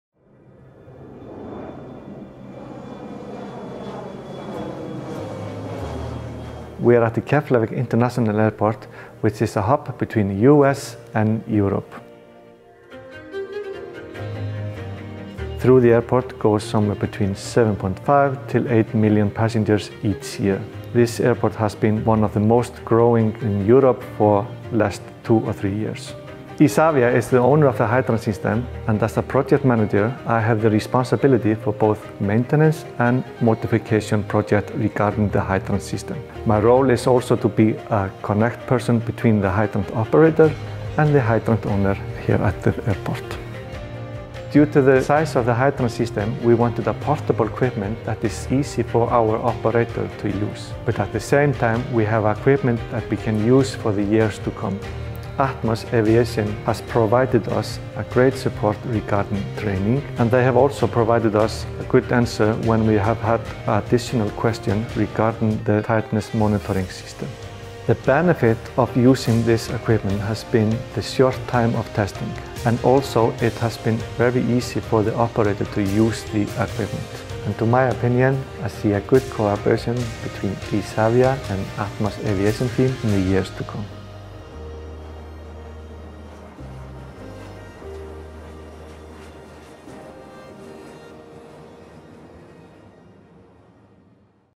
(EN) Atmos Portable Tightness Monitor at Keflavik International Airport
Discover how Atmos International's APTM system delivers results for a major hub between North American and Europe.